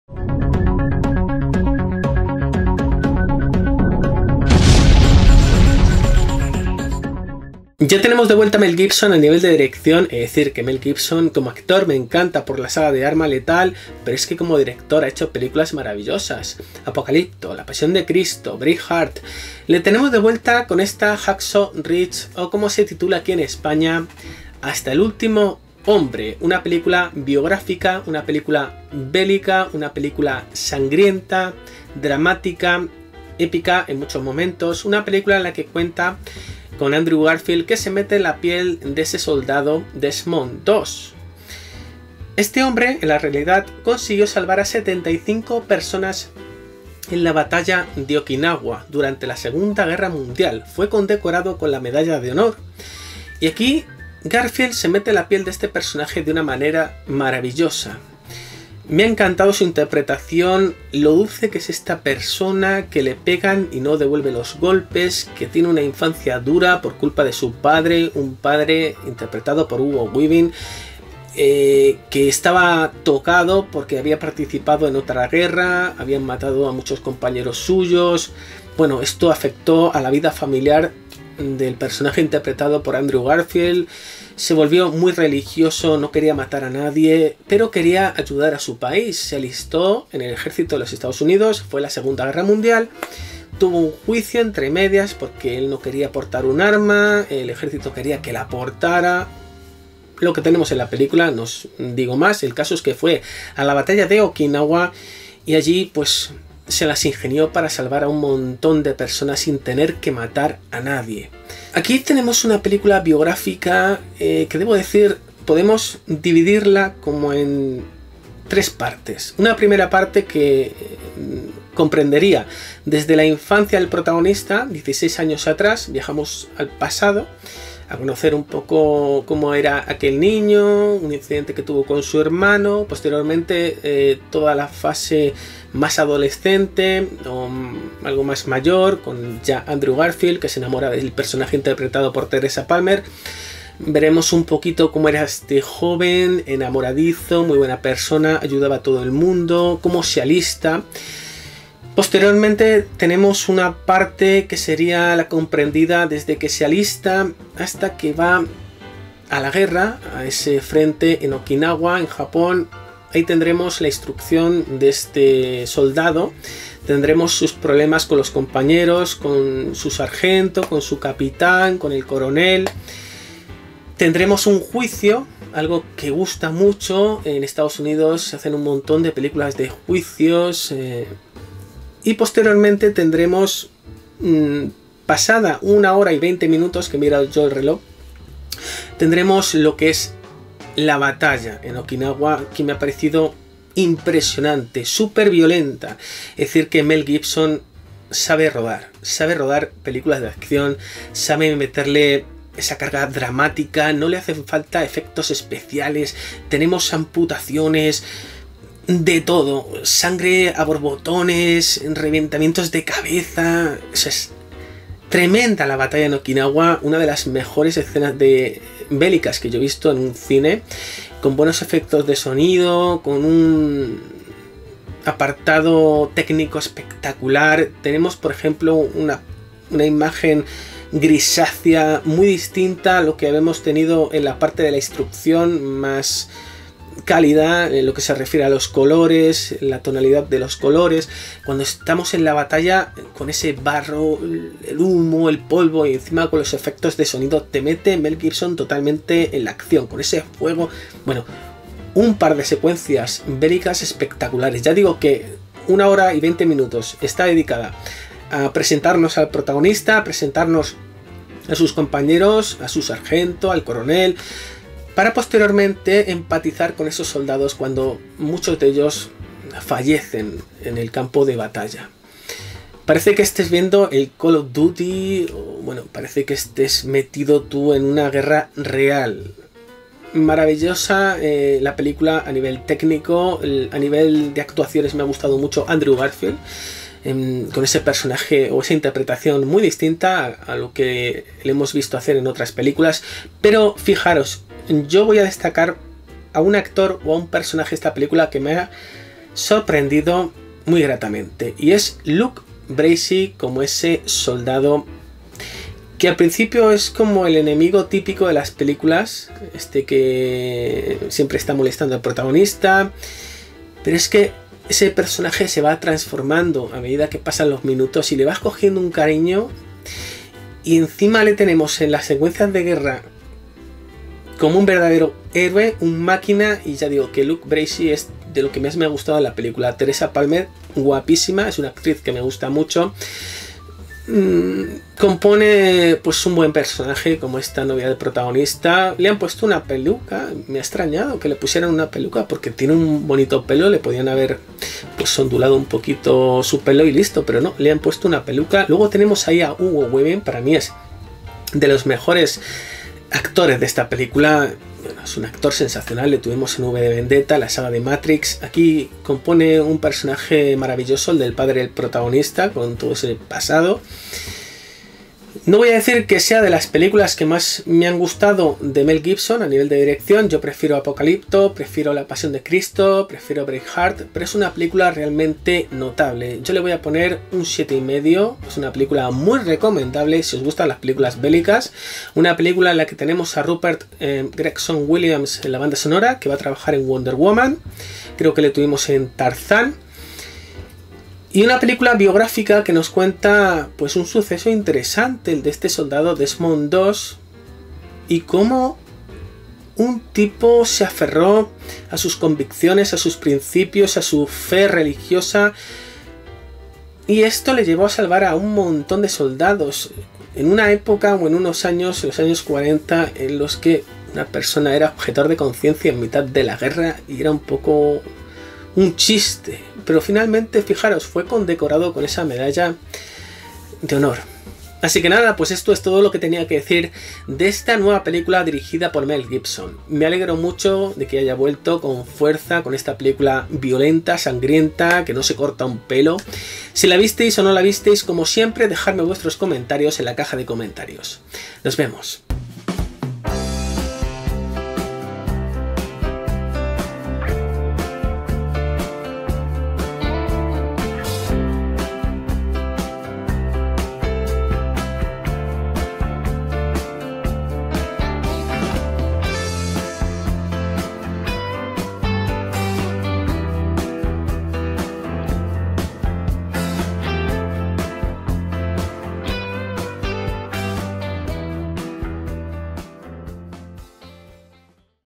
Hasta el último hombre - CRÍTICA - REVIEW - OPINIÓN - Hacksaw Ridge - John Doe
Cine: crítica, review, vídeo-crítica, análisis de la película Hasta el último hombre - Hacksaw Ridge.

Dirección: Mel Gibson.
Guión: Robert Schenkkan, Randall Wallace, Andrew Knight.
Reparto: Andrew Garfield, Vince Vaughn, Hugo Weaving, Teresa Palmer, Sam Worthington, Luke Bracey, Rachel Griffiths, Richard Roxburgh, Matt Nable, Nathaniel Buzolic, Ryan Corr, Goran D. Kleut, Firass Dirani, Milo Gibson y Ben O'Tool.
Banda Sonora: Rupert Gregson-Williams.
Fotografía: Simon Duggan.
Duración: 131 minutos.